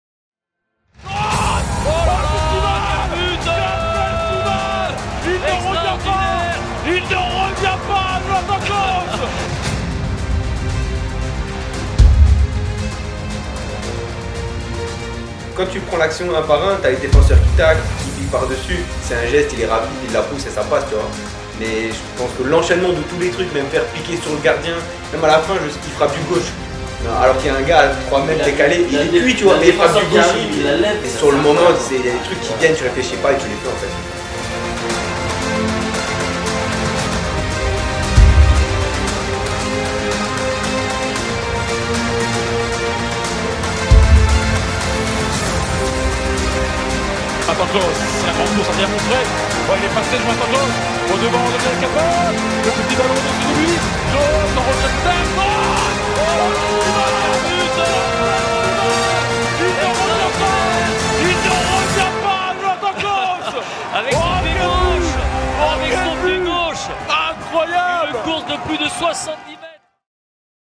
Teaser sur le reportage du footballeur professionnel Jonathan CLAUSS
Début du reportage sur le footballeur professionnel de 2eme divison allemande Jonathan CLAUSS.

De son échec à Strasbourg, en passant par son incroyable but en coupe de France jusqu'à sa signature à BIELEFELD en seconde division allemande, nous retraçons son parcours.. découvrez l'histoire de ce joueur atypique.

Production: L'amateur sportif
Réalisation: Benjamin Genestoux